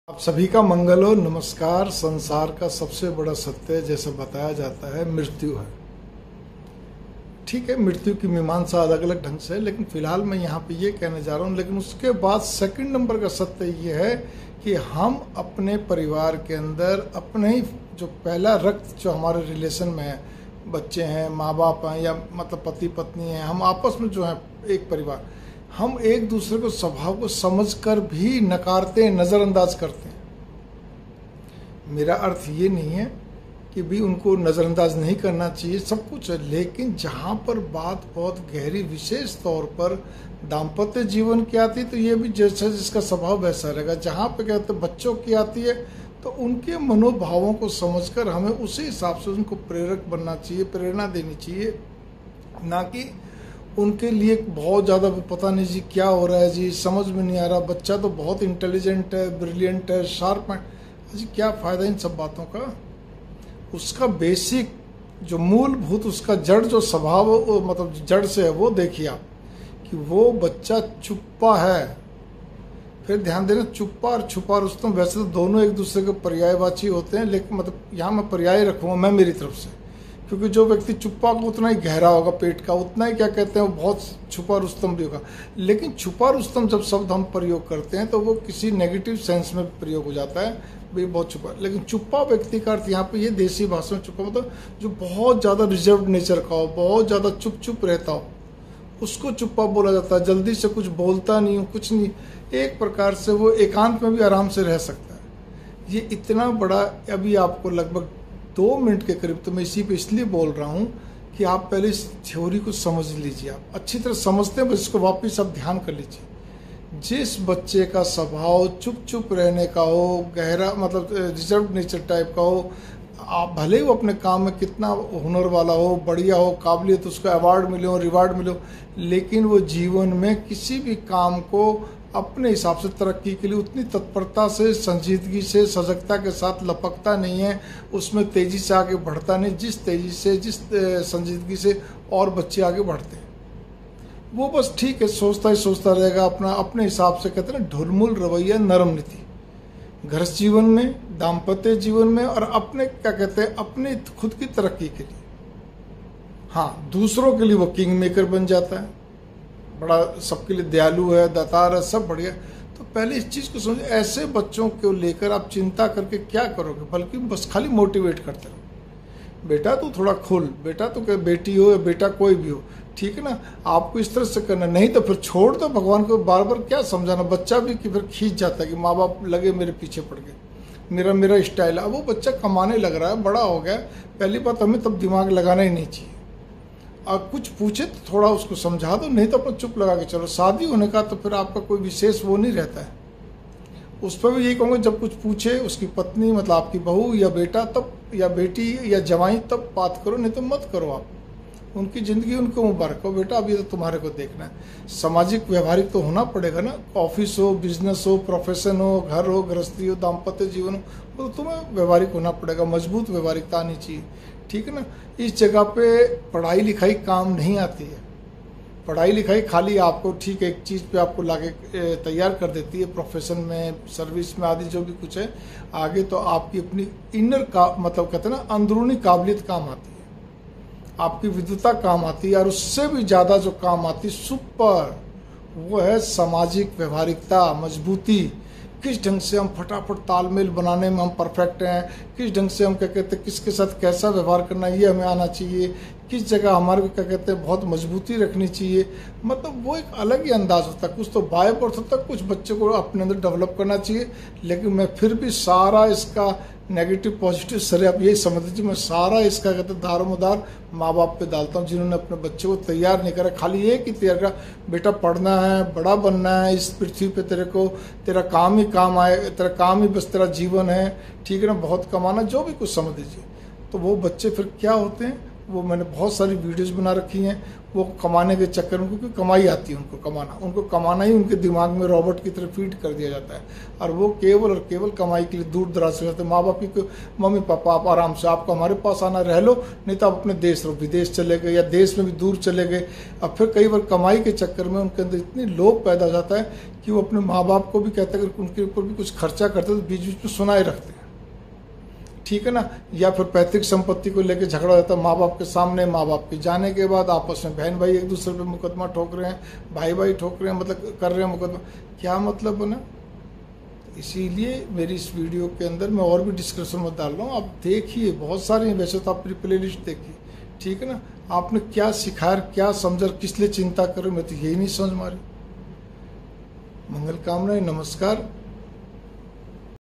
बच्चों को लेकर व्यर्थ में तनाव नहीं रखे वरन उसकी तासीर को समझे.? 1284
1284 बच्चों को लेकर व्यर्थ में तनाव नहीं रखे वरन उसकी तासीर को समझे.?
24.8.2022 Note-Please use Earphone for clear voice
(Ravinder Lakhotia Author Social Councellor & Thinker)